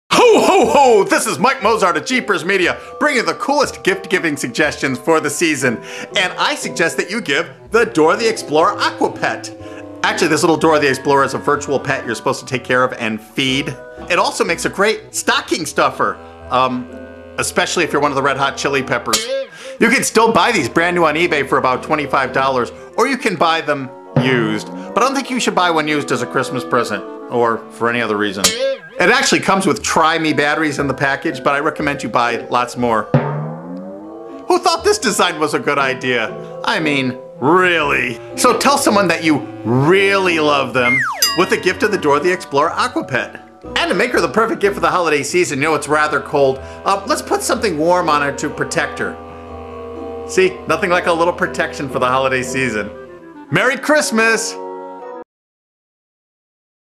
Funny Video , Dora the Explorer Christmas Video Review Mike Mozart
Funny Video of the Fail Toys Dora the Explorer Aquapet Appears in Another Funny lol video! The Epic Failure Toy Dora the Explorer Aquapet product for Christmas! A Funny Christmas Video card to send! CLICK HERE TO SEND AS A VIDEO GREETING CARD!
Another funny toy product review by Mike Mozart of JeepersMedia on You Tube